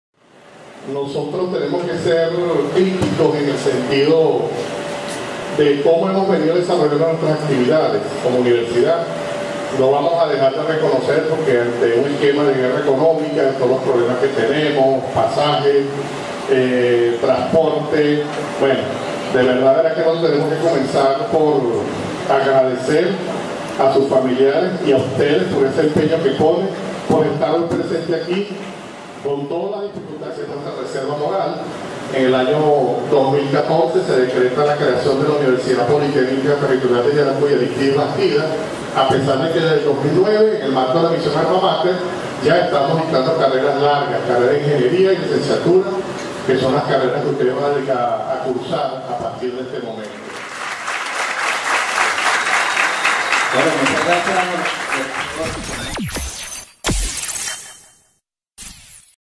¡BIENVENIDAS Y BIENVENIDOS!
En el marco de la profundización de los estudios universitarios municipalizados, el Ing. Víctor Gamarra, rector de la UPT de Yaracuy "Arístides Bastidas" recibe y brinda solidaria bienvenida al contingente de estudiantes del nuevo ingreso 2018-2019.